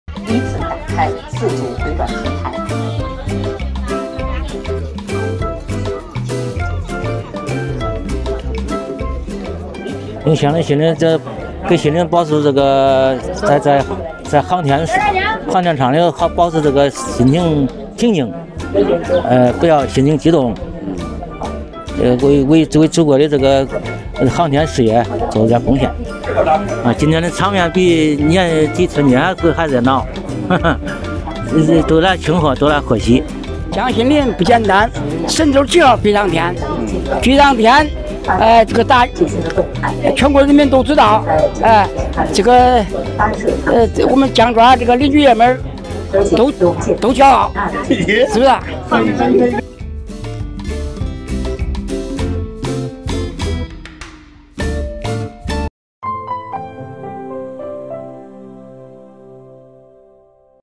探访航天员江新林河南杞县老家：热闹似过年
10月26日，记者赶到航天员江新林的老家河南杞县圉镇镇乔庙村看到，四方宾朋和乡邻闻讯赶来道贺，让昔日的农家小院顿时热闹起来，充满了欢声笑语。人们围绕在电视机前，同航天员江新林父母一起见证神舟十七号载人飞船出征。江新林的父亲江孝涛不停地忙着招呼客人，他说：“比春节过年还热闹”。